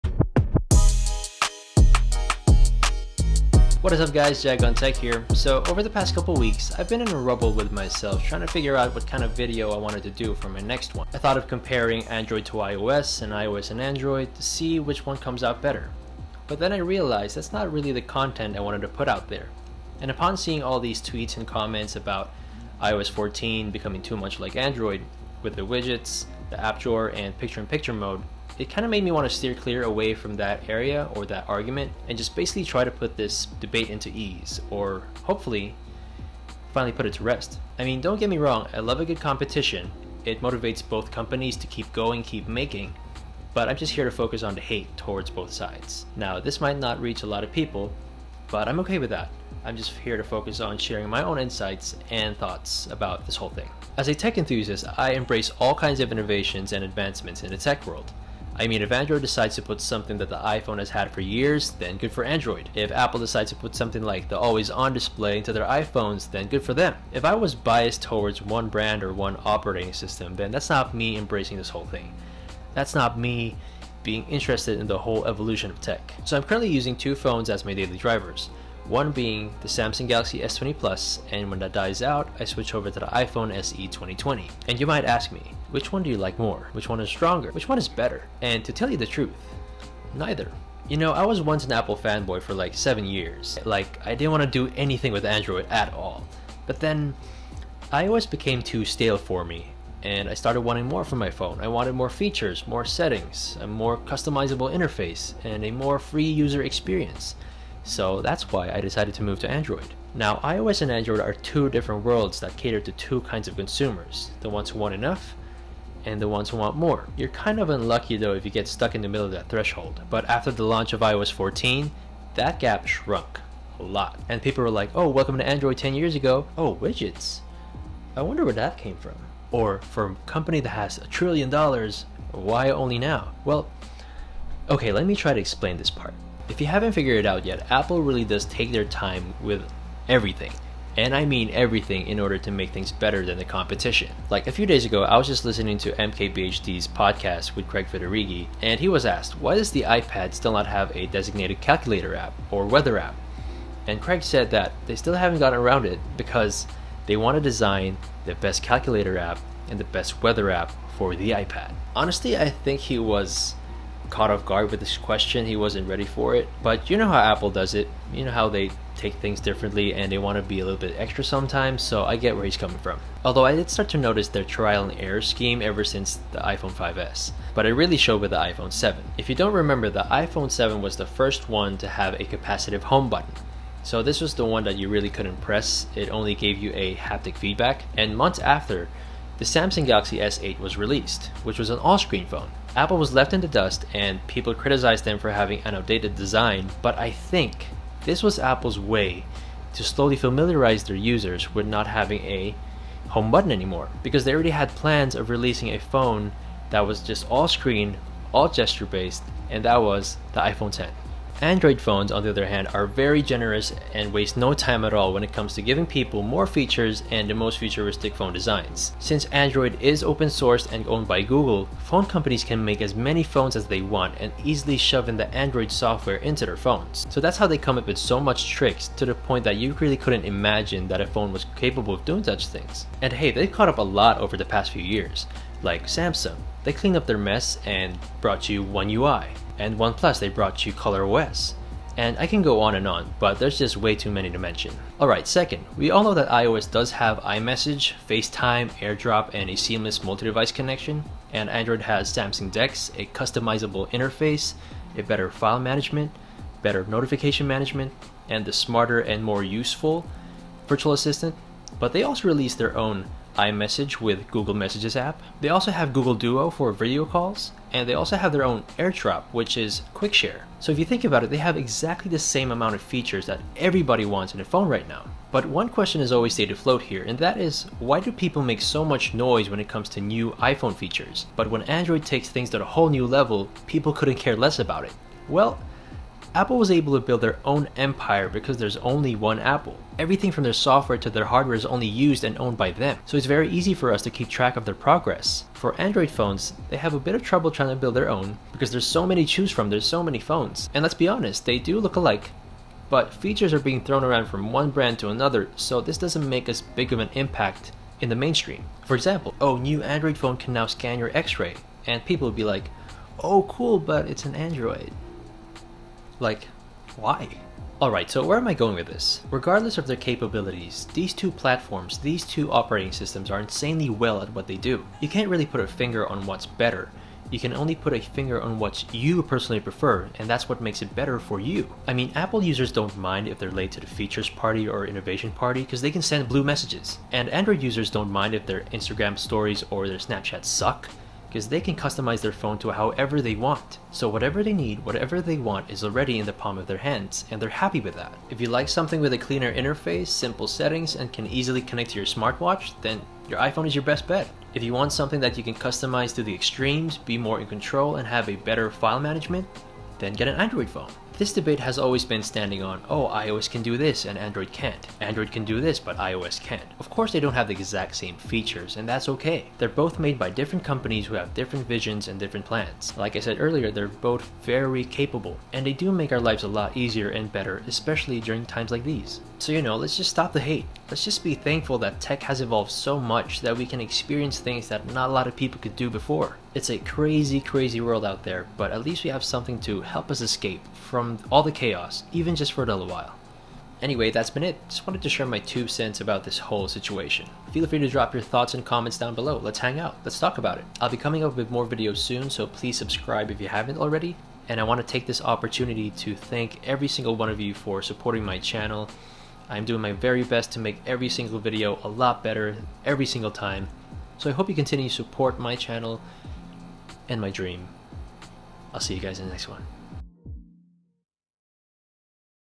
iOS and Android | Is There Really A Better One?
Yes, these two have a lot more similarities now after iOS 14. Can we stop there?